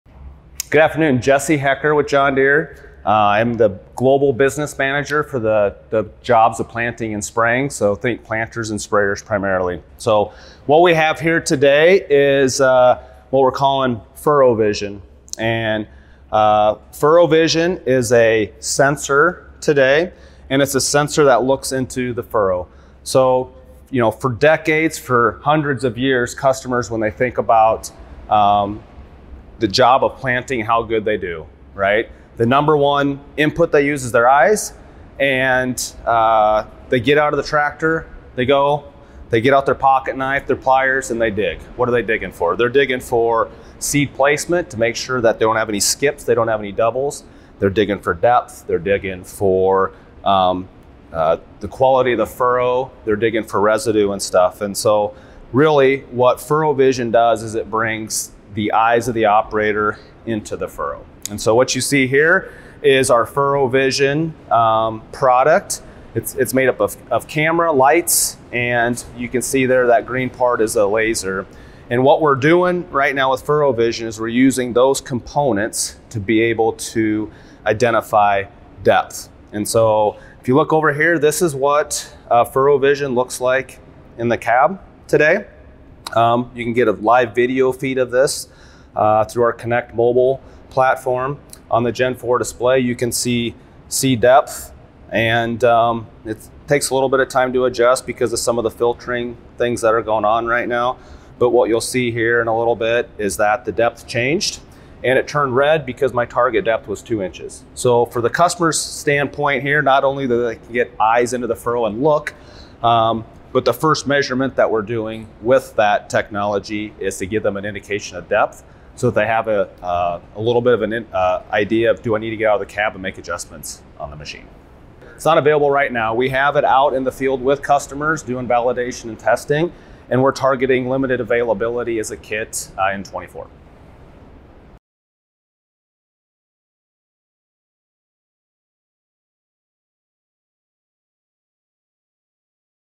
Deere's FurrowVision Brings Eyes of Operator into the Furrow
Jesse Haecker, Global Business Manager for Planting, Spraying & Nurture Business with John Deere showcases the company's new planter performance upgrade, FurrowVision. This new technology will allow growers to see exactly what is happening in the furrow without having to get out of the cab. The sensor employs lights, lasers and cameras to display a live feed of the furrow's seed depth and alert the farmer when it is not meeting their specifications. While currently in the validation and testing stage of development, FurrowVision is planned for limited availability as a kit as soon as 2024.